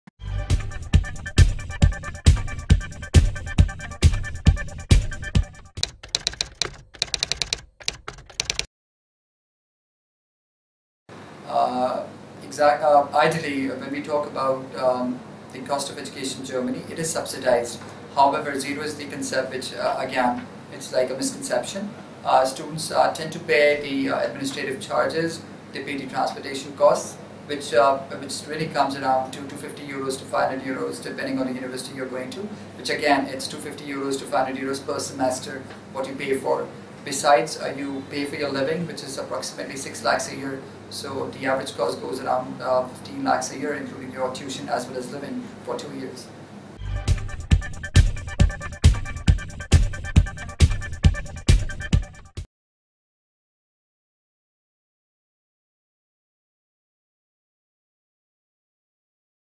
Do German universities really charge no tuition fees?
Get know what tuition fees in Gremany comprise of, Also experts advice on inclusion and exlcusion of tutition fees.